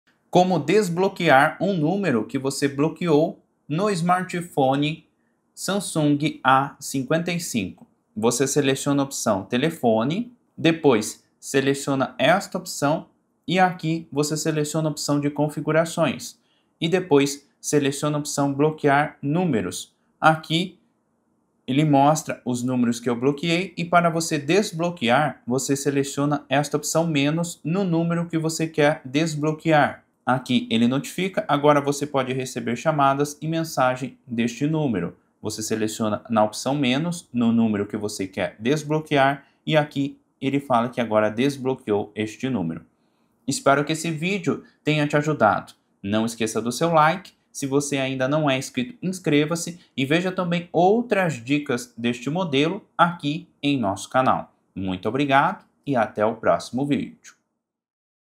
SAMSUNG A55 Como desbloquear um número bloqueado
Neste vídeo vou te ensinar como desbloquear um número bloqueado no Samsung A55

Samsung A55
🛒 Samsung Galaxy A55 -